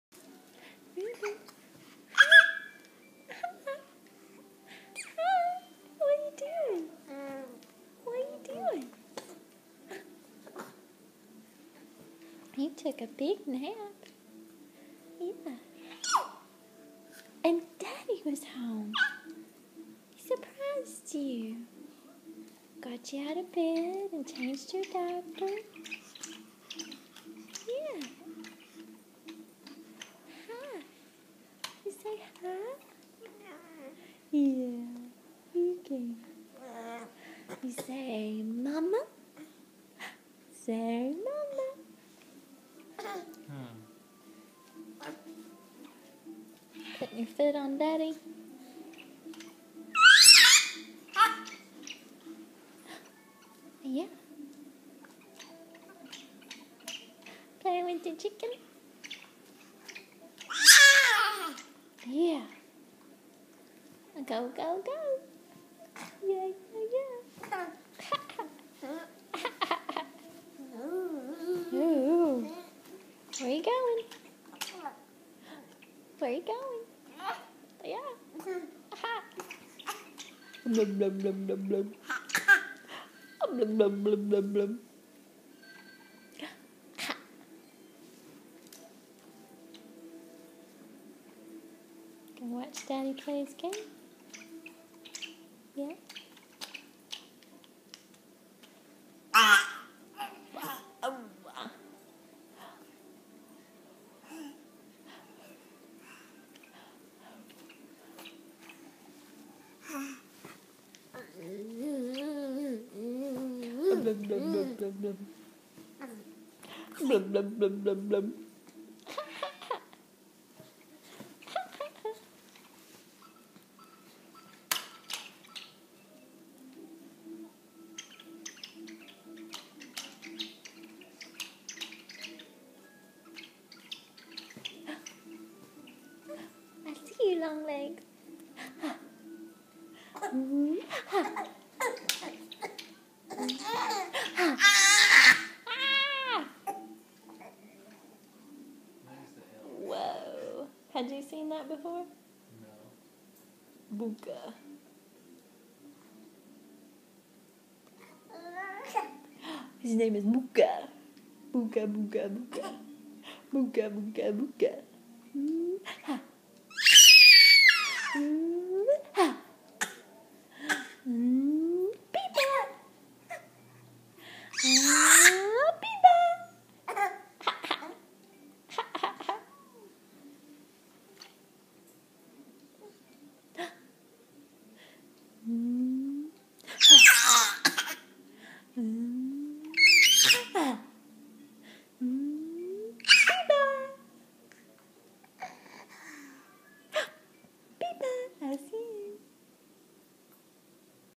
Snow day...sorta
Josh's overtime this week paid off. He got to come home early today, the day it is snowing everywhere but here.